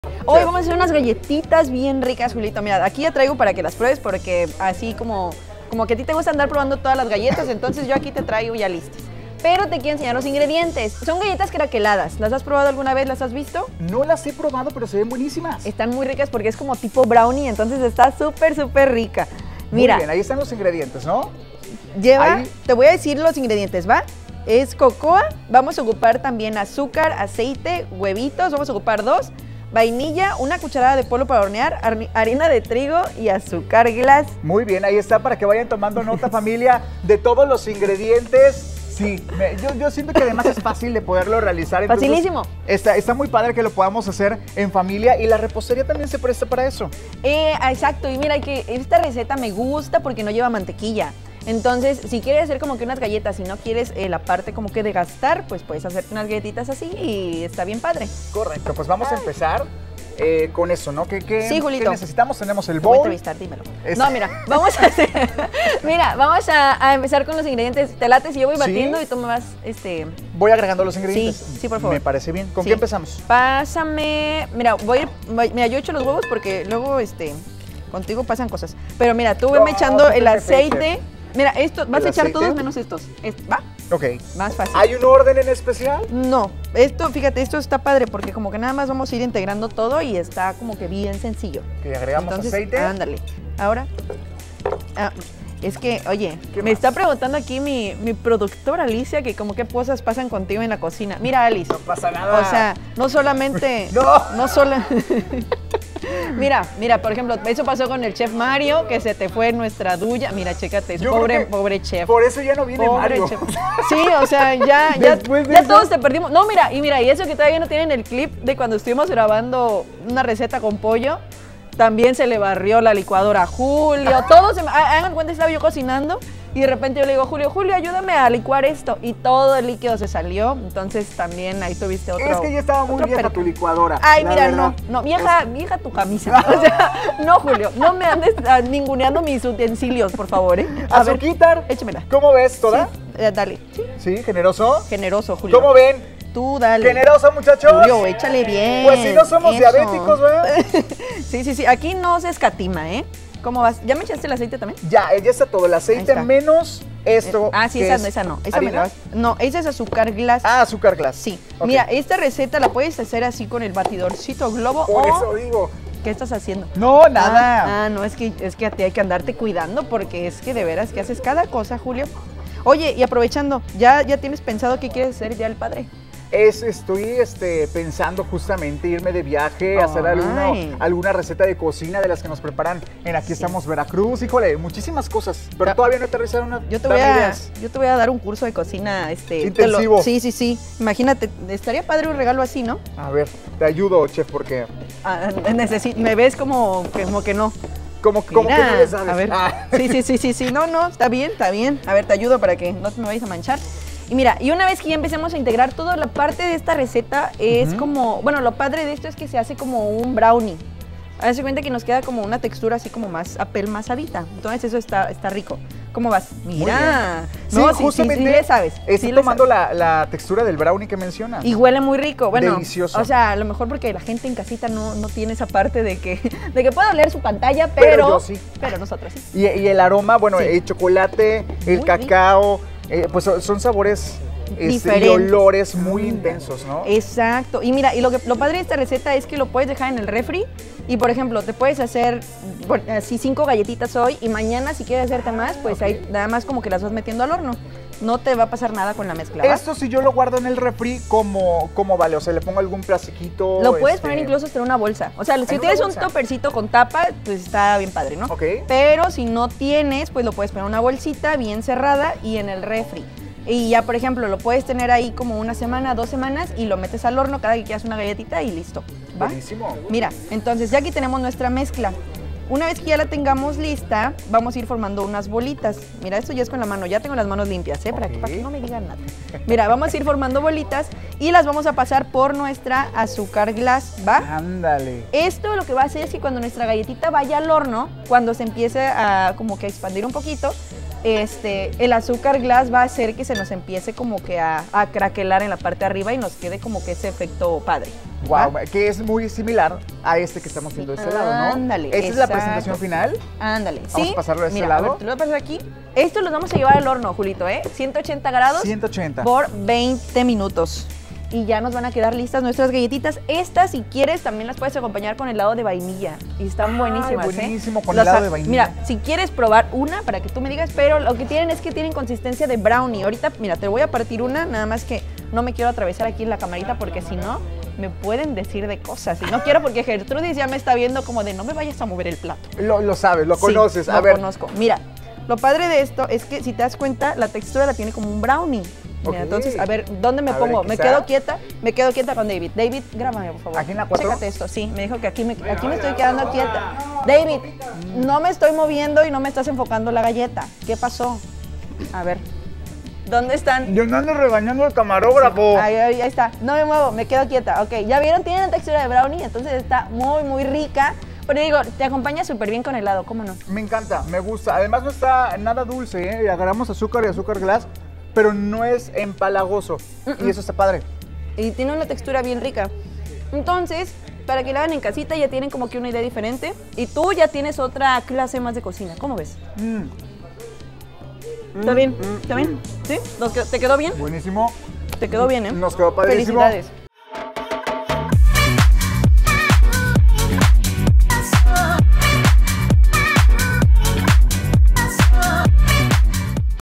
Receta para preparar choco galletas craqueladas.
Cocina | La chef Valeria Ochoa toma medidas para que no arruine Julio la receta "Choco galletas craqueladas".